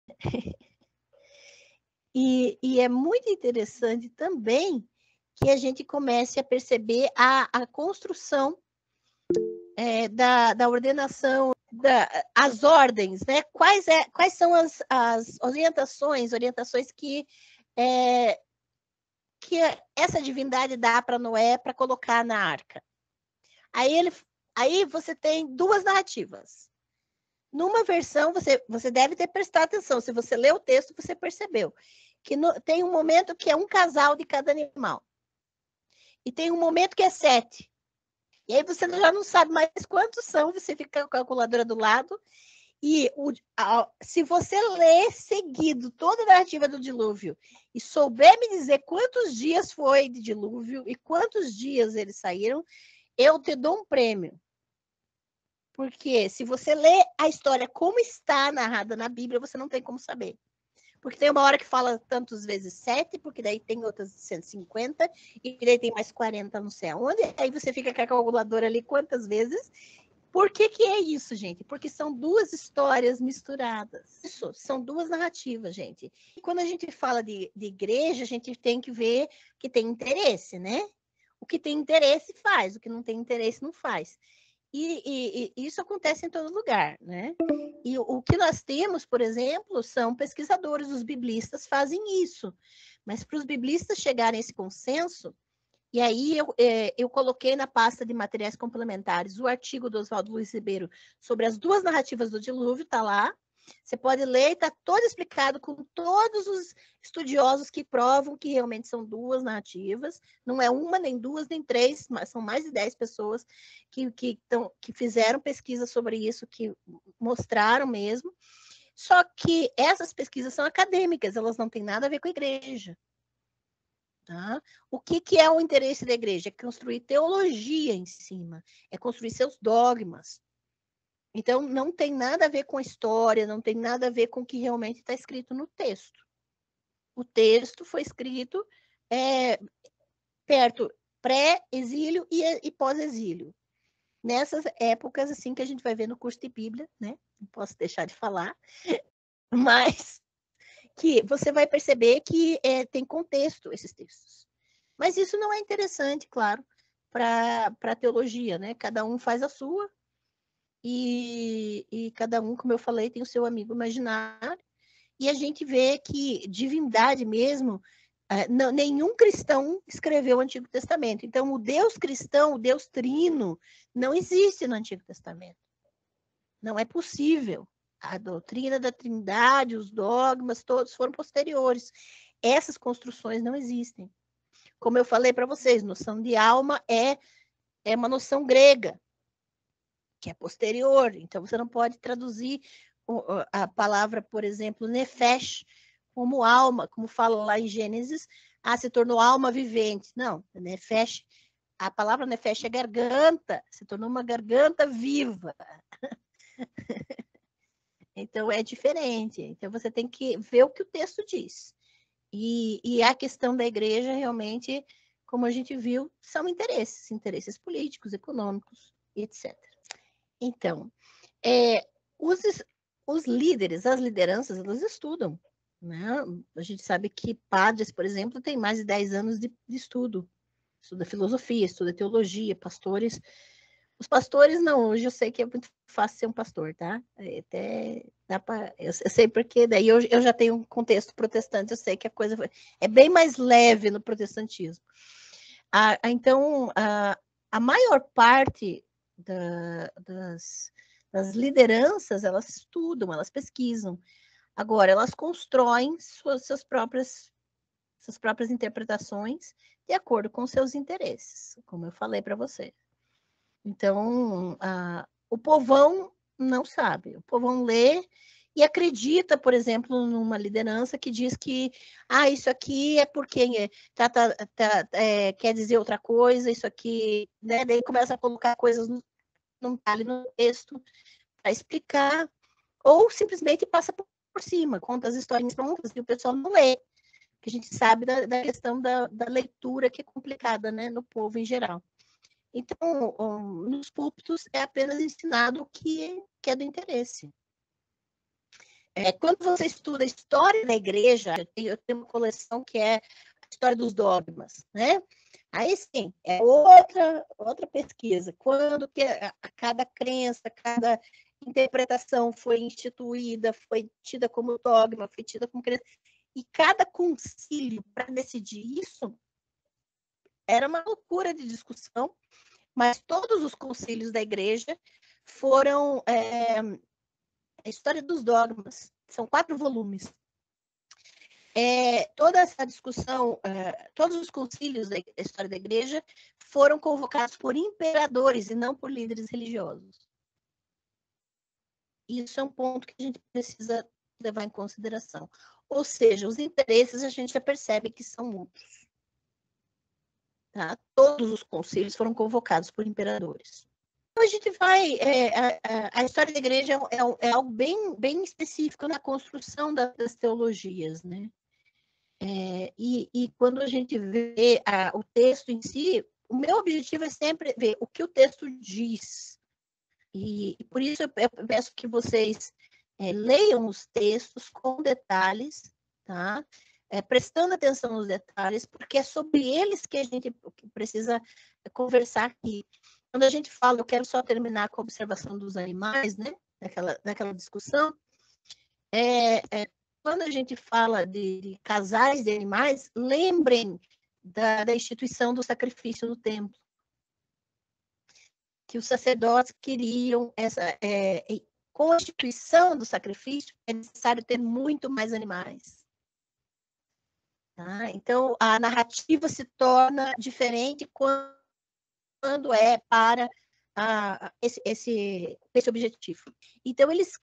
Pentateuco Aula 3 - As duas narrativas do dilúvio e como os cristianismos trabalham com elas
Referências:
Hipótese sobre a introdução em Judá do sacrifício expiatório Gn 2,4b-3,24; 4,1-16 e 6,5- 9,17* como “eixo estrutural” de Gênesis 1-11

As duas narrativas bíblicas do dilúvio. A função social do mito na Bíblia Hebraica.

Trecho do curso "Exegese do Antigo Testamento: Heroínas, vilões, sedutores ou vítimas? Personagens e caracterização no Pentateuco", ministrado de forma online em janeiro de 2022.

FORMAS DE PAGAMENTO:
2. PicPay: @angelanatel
3. Mercado Pago (nesse caso acrescentando 5 reais ao valor total)